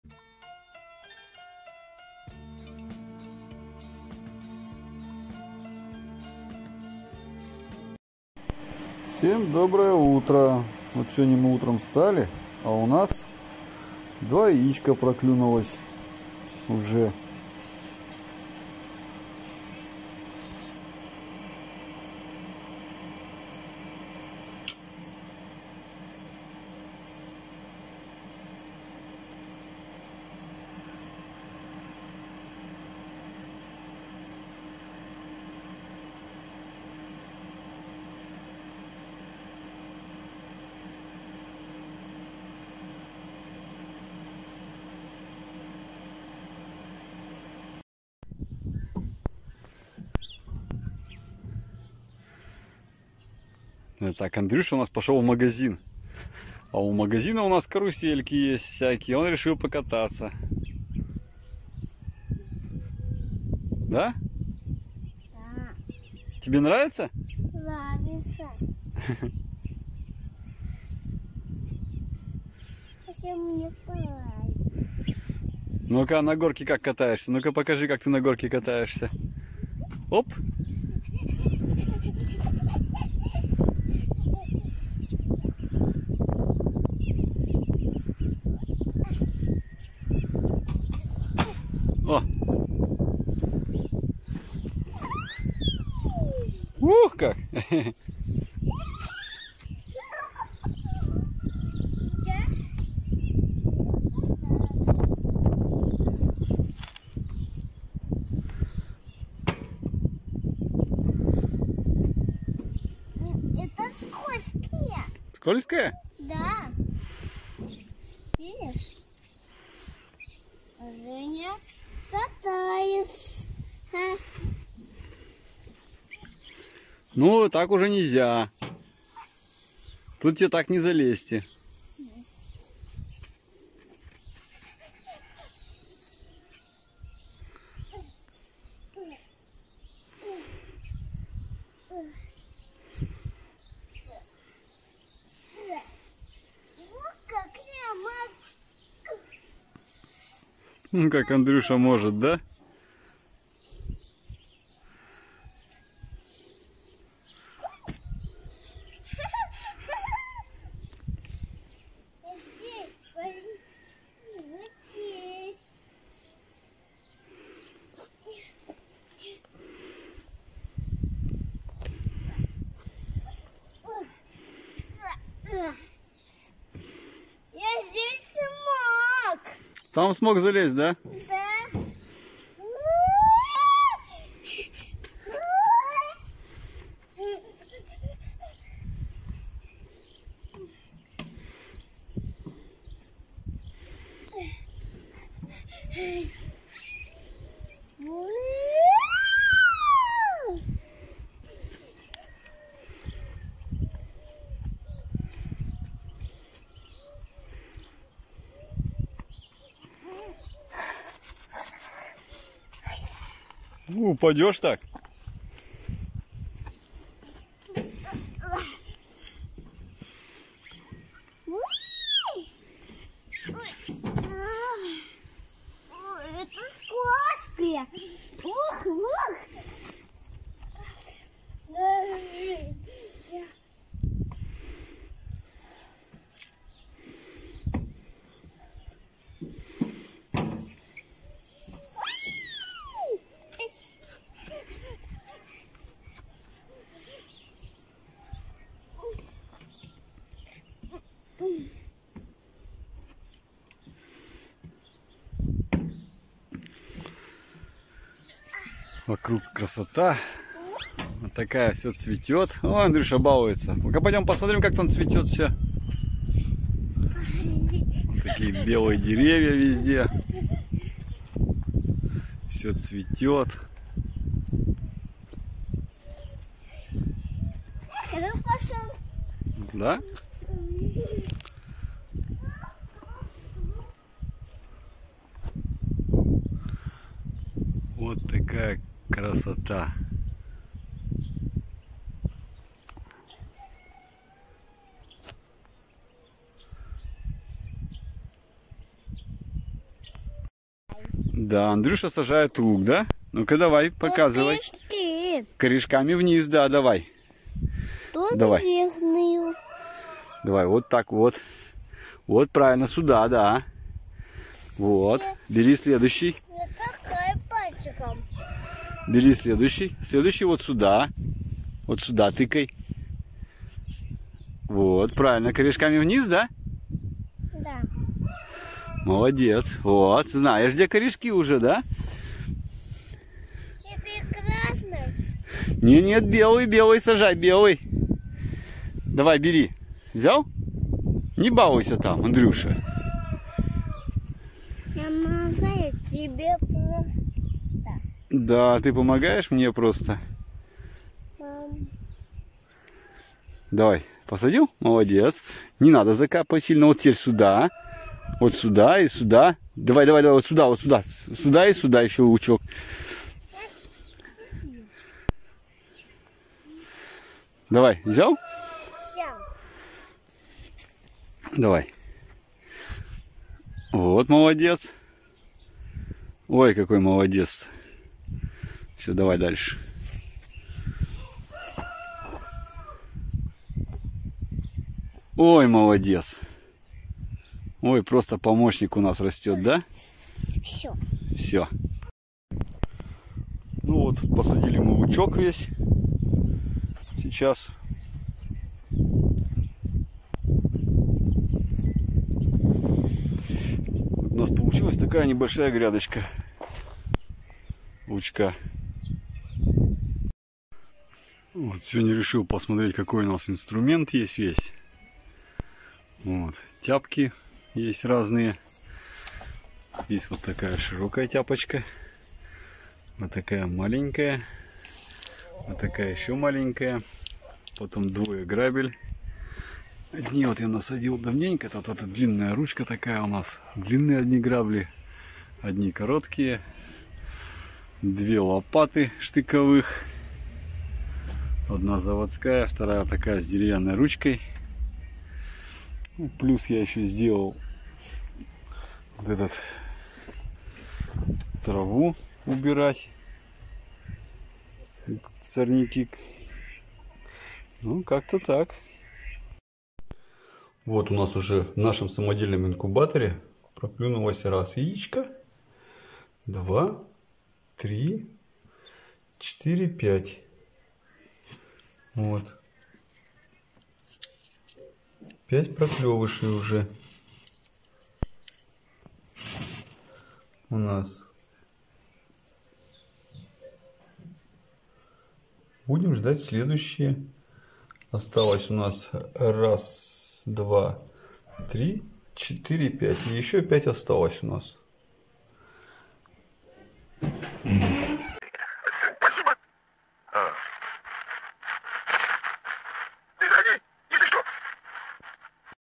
Деревенские будни, вылупляются цыплята
гуляем, сажаем с Андреем, вылупляются цыплята